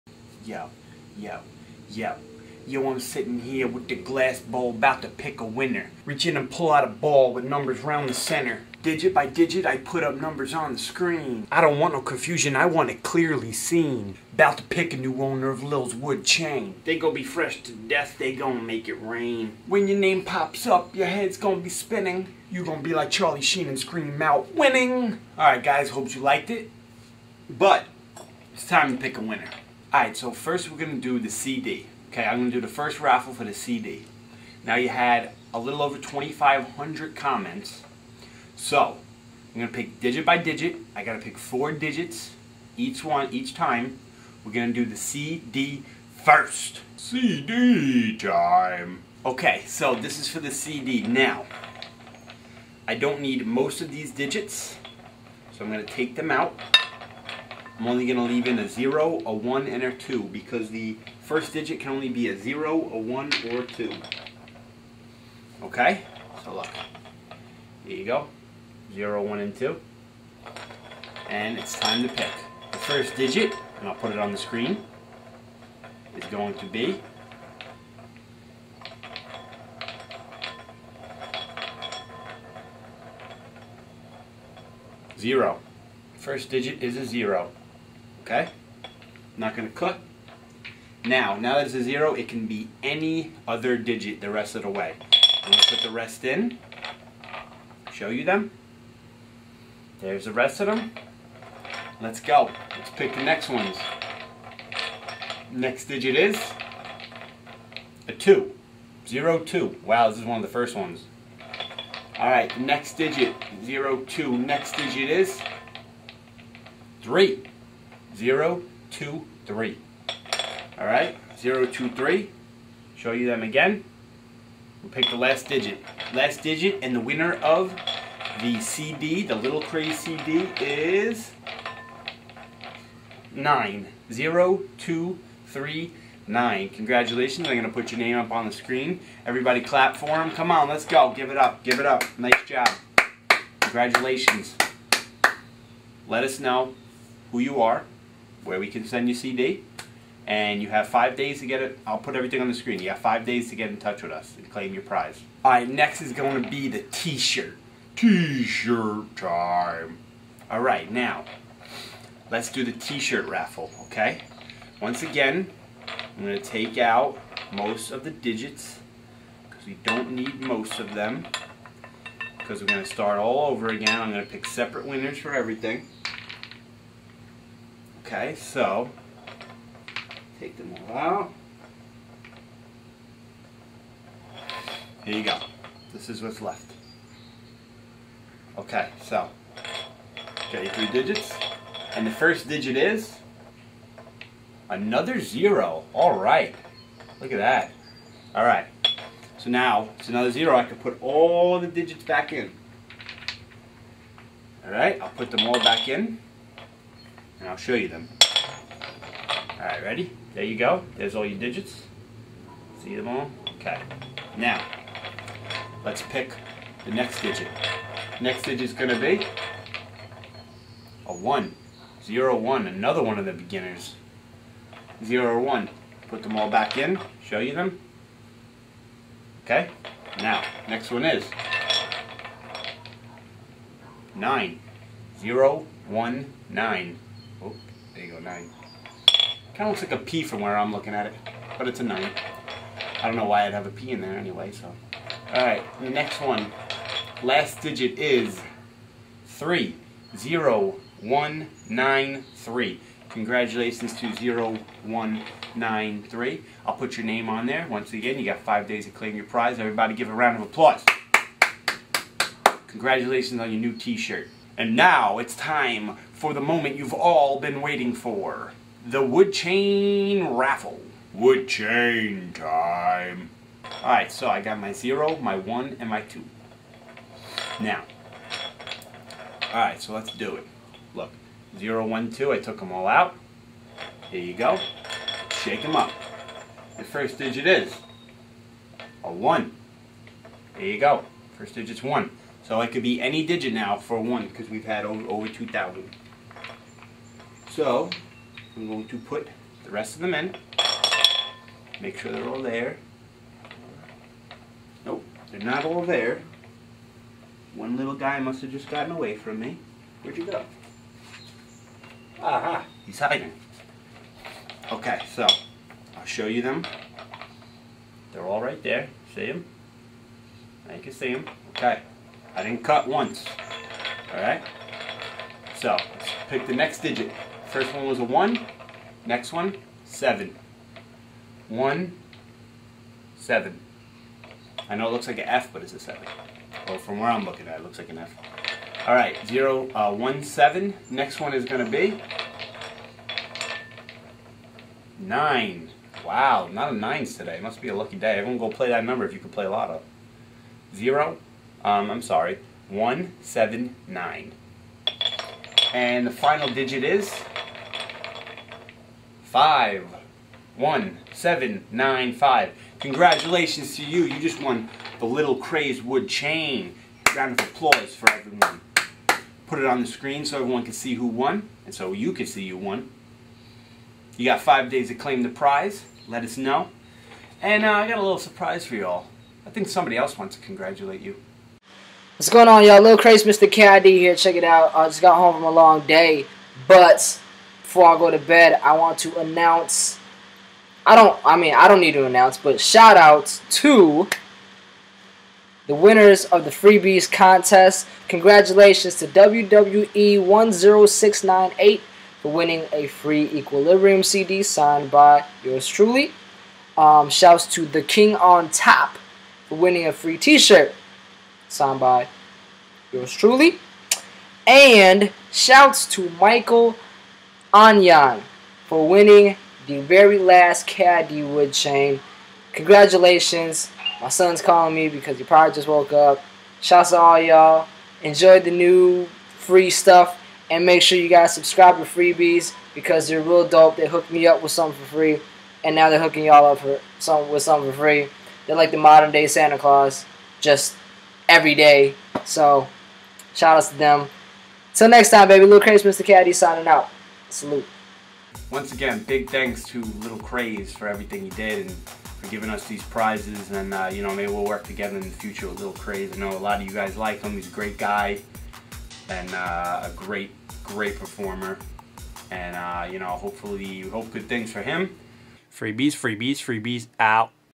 LilCrazed Giveaway - Winner Announcement!! (feat. LilCrazed)
The winner video is finally here. Special guest appearance after the winners are announced! Remember you have till Saturday to claim your prizes. Good Luck in the future contests.